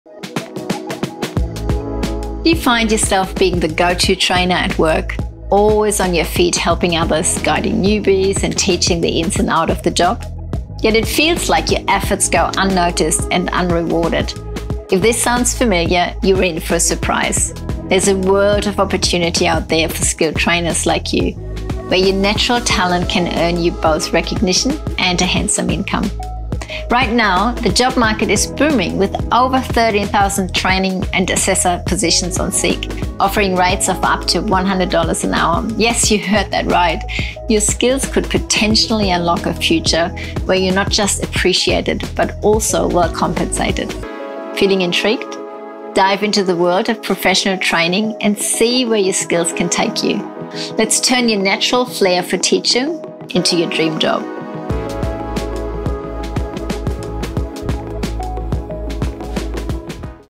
From Unnoticed to Unstoppable: Unlock Your Training Talent!
Are you the go-to trainer at work, always helping others and guiding newbies, but feel like your hard work goes unnoticed? 🤔 Don't worry, because in this video, we're going to show you how to unlock your training talent and take your career to the next level! 💪

In today's job market, there's a world of opportunities waiting for skilled trainers like you. Did you know that there are over 13,000 Trainer and Assessor positions available on Seek? 🌍 And the best part is, some of these positions offer rates of up to $100 an hour! 💰 So, if you've been feeling unappreciated and under-compensated, it's time to change that.

Imagine a future where your natural talent for teaching not only earns you recognition but also a handsome income. Sounds intriguing, right? 🤩 Well, it's possible! With the booming job market, now is the perfect time to dive into the world of professional training and see where your skills can take you.

In this video, we'll guide you through the steps to unlock your training talent and turn it into your dream job. From finding the right opportunities to showcasing your skills, we've got you covered. 🎓 So, whether you're a seasoned trainer or just starting out, this video is for you.

Don't let your efforts go unnoticed any longer. It's time to finally be appreciated for your expertise and be well-compensated for your hard work. 💪 So, what are you waiting for? Join us on this journey and discover the exciting possibilities that await you in the world of professional training! 🌟